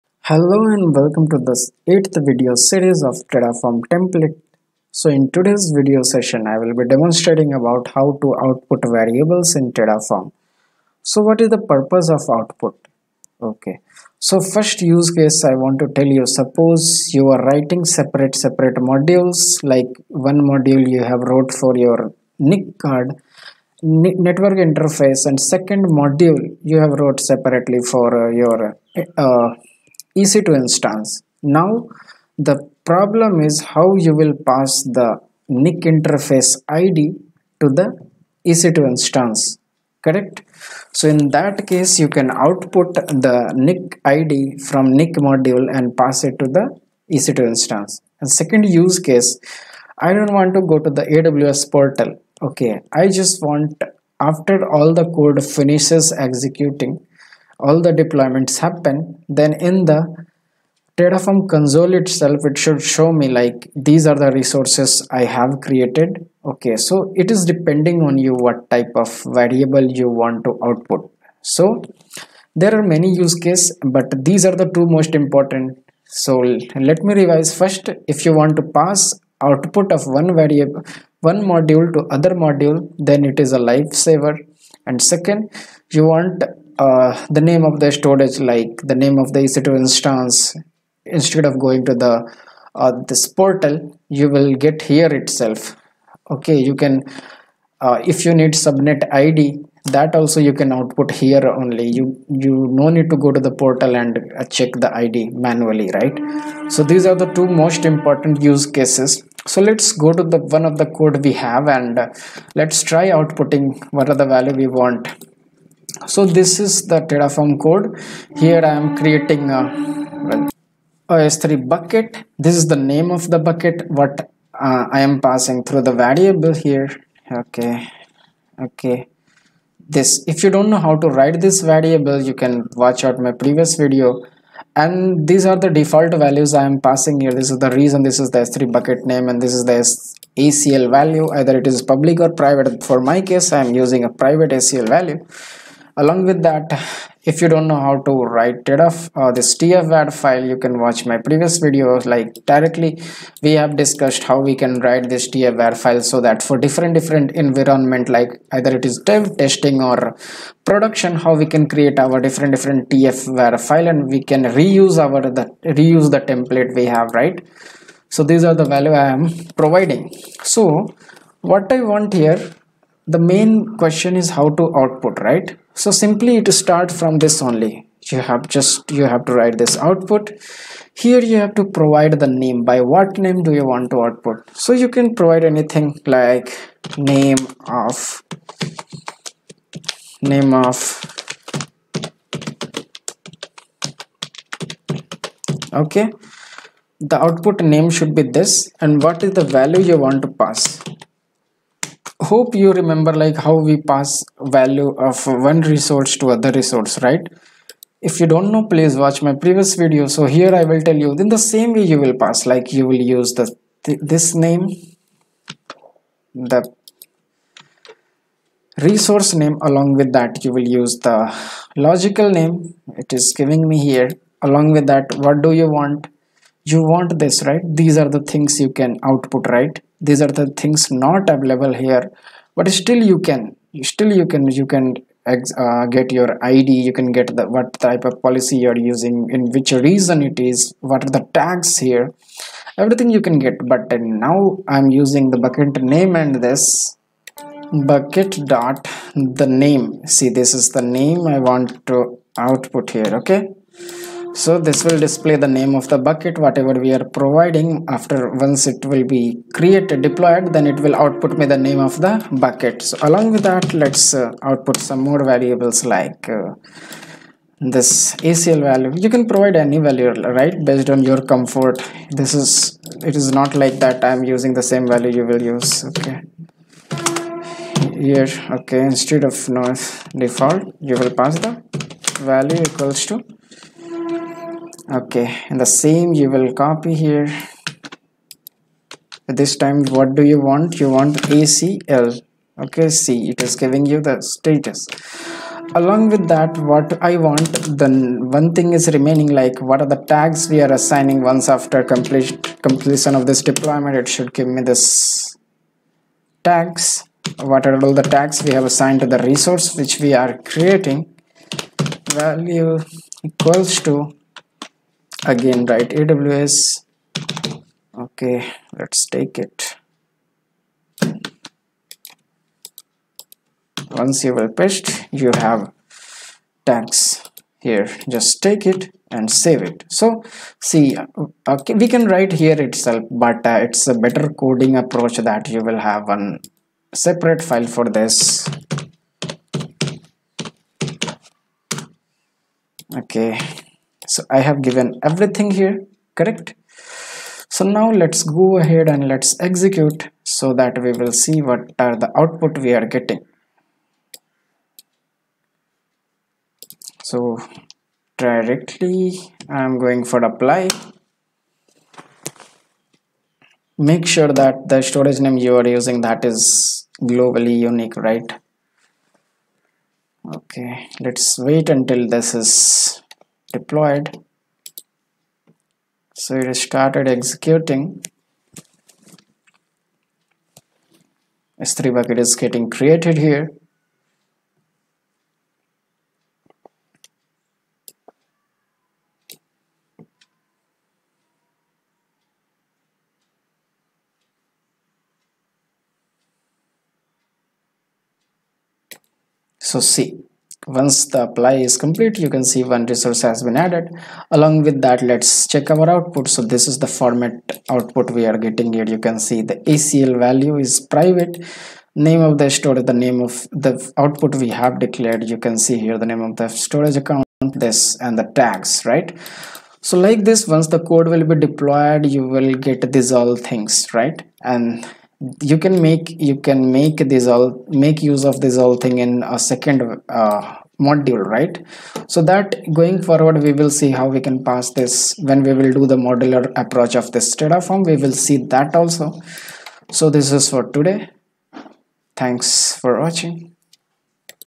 How to Output Variables in terraform | HandsOn Lab | Terraform explained - 08 | Learn Like a pro
Output Values
Output values make information about your infrastructure available on the command line, and can expose information for other Terraform configurations to use. Output values are similar to return values in programming languages.